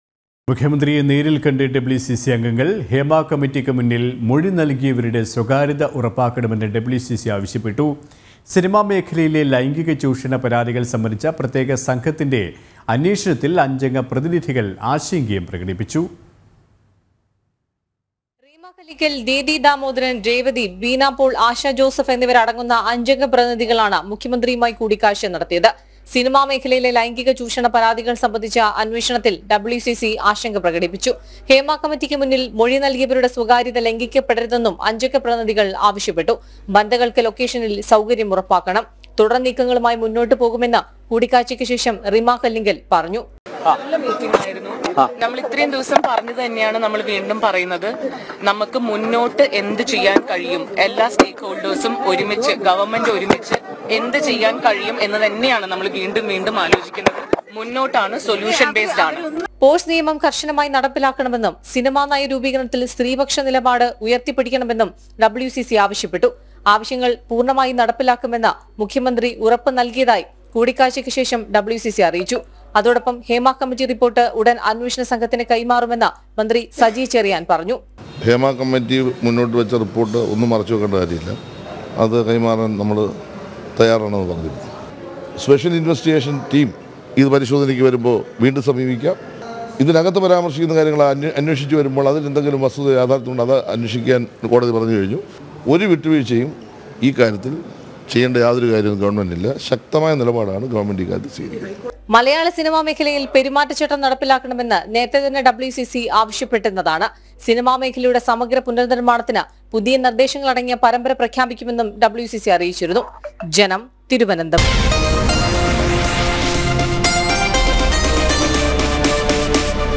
ഹേമ കമ്മിറ്റിക്ക് മൊഴി നൽകിയവരുടെ സ്വകാര്യത ഉറപ്പാക്കണമെന്ന് WCC
മുഖ്യമന്ത്രിയെ നേരിൽ കണ്ട് WCC അംഗങ്ങൾ. ഹേമ കമ്മിറ്റിക്ക് മുന്നിൽ മൊഴി നൽകിയവരുടെ സ്വകാര്യത ഉറപ്പാക്കണമെന്ന് WCC ആവശ്യപ്പെട്ടു. സിനിമാ മേഖലയിലെ ലൈംഗിക ചൂഷണ പരാതികൾ സംബന്ധിച്ച പ്രത്യേക സംഘത്തിന്‍റെ അന്വേഷണത്തിൽ 5 അംഗ പ്രതിനിധികൾ ആശങ്ക പ്രകടിപ്പിച്ചു.